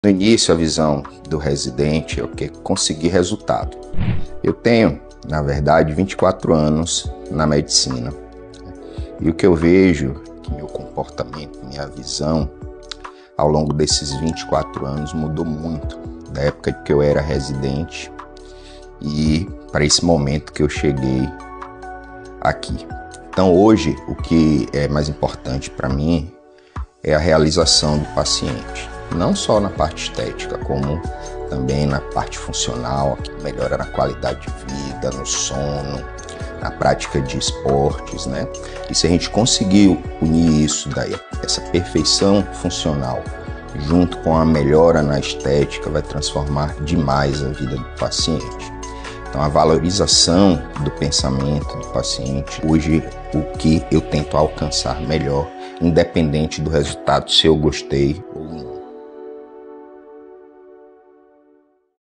O que eu aprendi em 2 décadas como cirurgião… |Dr Paulo Dórea
O que eu aprendi em 2 décadas como cirurgião vai muito além de prática e corpos… aprendi sobre humanidade e sobre pessoas também.🥰

Muito mudou desde a época que eu era residente, e queria poder compartilhar um pouco da minha história com vocês.

Que muitas outras vidas possam ser mudadas com a cirurgia plástica e a parceria que construímos nesse processo!❤️